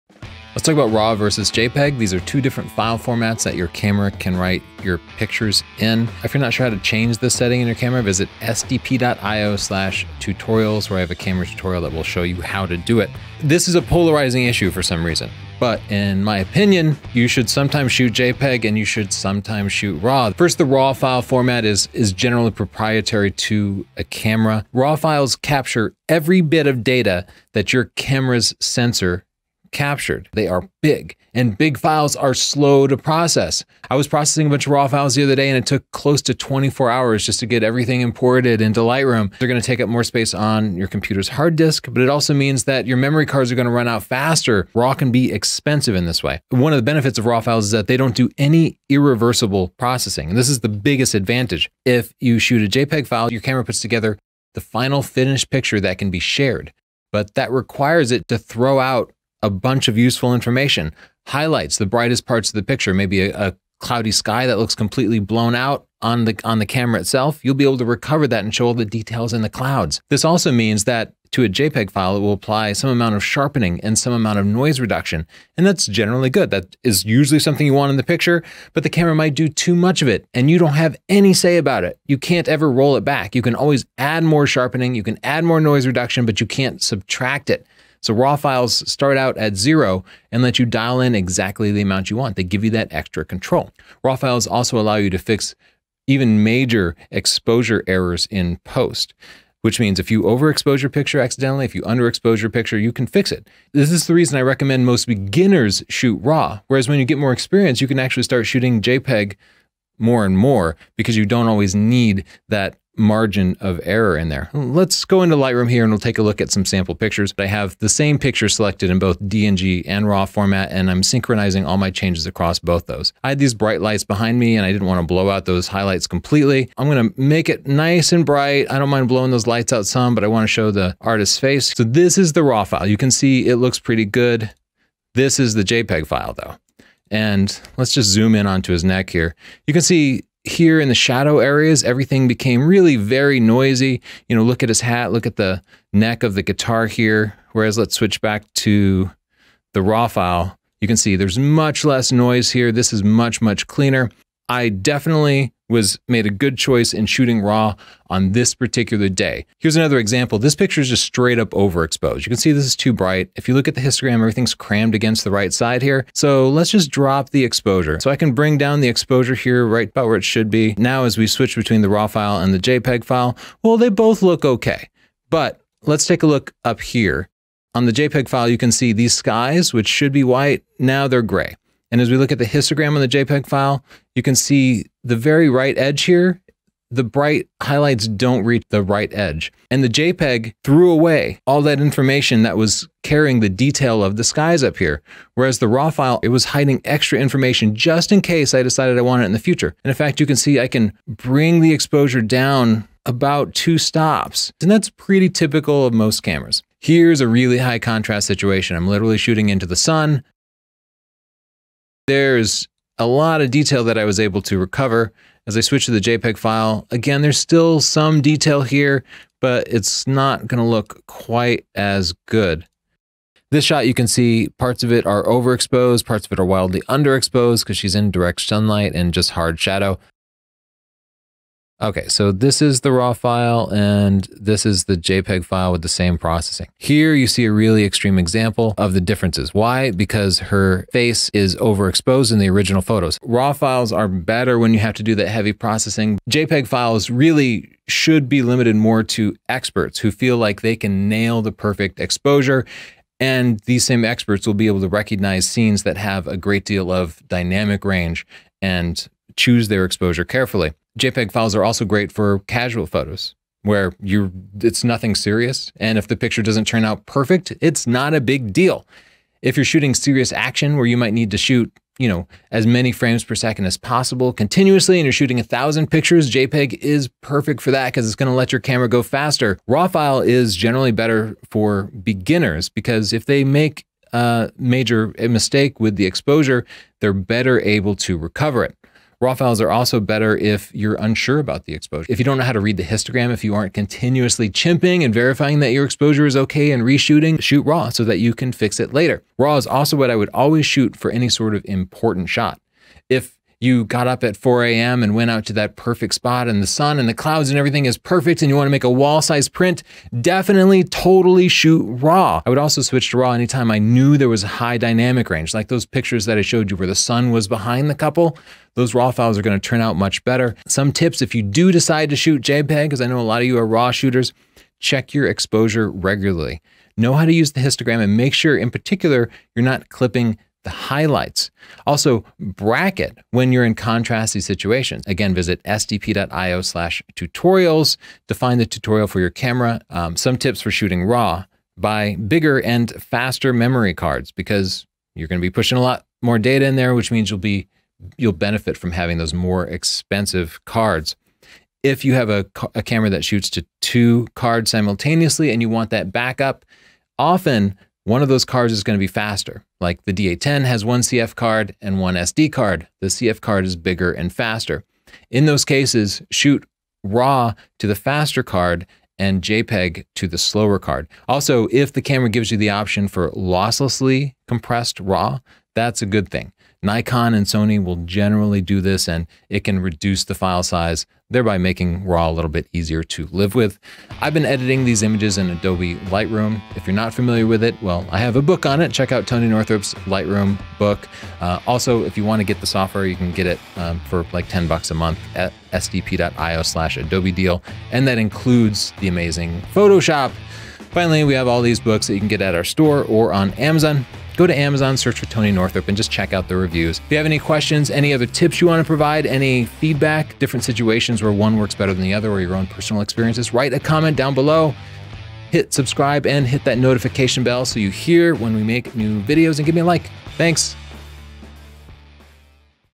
Raw vs JPG: Image Quality vs Speed, when to use each
Hi! If you're here because of Jared, read this before you comment:

Summary: Raw is better for image quality, but JPG files are quicker to shoot and process. Raw is the right choice for most photographers in most scenarios. I almost always shoot raw or raw + JPG. JPG might be a better choice for photographers taking a large number of pictures or those with limited storage.

The main point Jared and many of his followers seem to be missing: Raw might be right for YOU all the time, but other photographers have different cameras and shooting requirements. SO MANY parents are shooting their kids sports with a T6, which buffers after only 6 raw shots, and takes several seconds to clear. Shooting JPG will reduce the buffering.

Storage: a 4TB external drive is $100. Pretty cheap! But it's also really slow. The m.2 drive I use is $1,200 for 2TB, however... not so cheap! Plus, every photo needs an offsite backup, which generally means a cloud storage service, which costs about $10/TB/mo... and that adds up over the years! 20,000 5DS-R images over 10 years will cost you about $1,200 *just for the backup*. I know "unlimited" backup solutions exist; I've used two that changed their policy, because "Their Ain't No Such Thing As A Free Lunch'.

Re: my 24 hour import, that's based on me importing a few thousand 50 MP 5DS-R files into LR, and my standard import process includes rendering Smart Previews, 1:1 Previews, and converting to DNG. That gives me the best performance, but yeah, it takes a LONG TIME.

Another important point: JPG files ARE editable... as I demonstrate in this video. You can recover shadows, fix white balance, and much more. Raw files are always more editable, so when the raw format doesn't impede your shooting, choose raw!